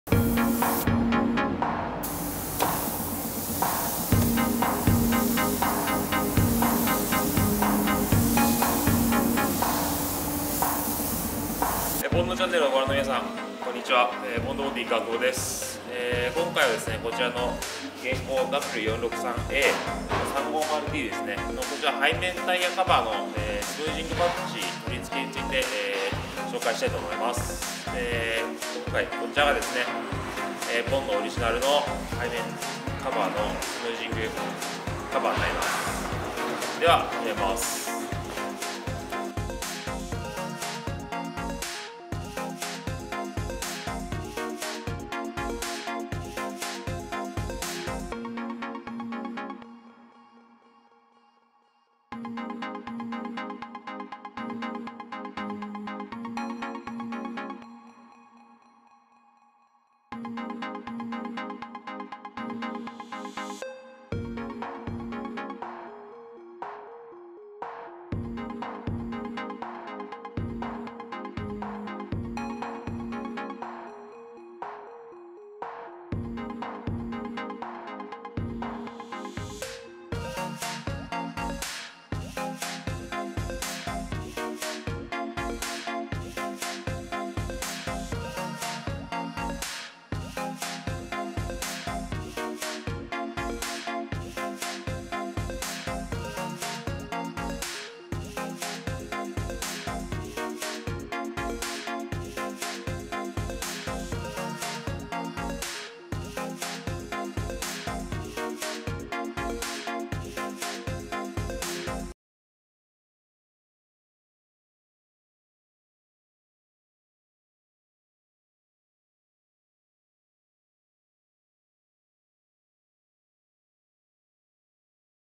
[商品紹介] BONDオリジナル!! 新型ゲレンデ背面タイヤスムージングキャップ
こちらの商品で新型Gクラスの背面タイヤレスをスマートに演出する事が可能です。

bond body
〒353-0001
埼玉県志木市上宗岡3-7-6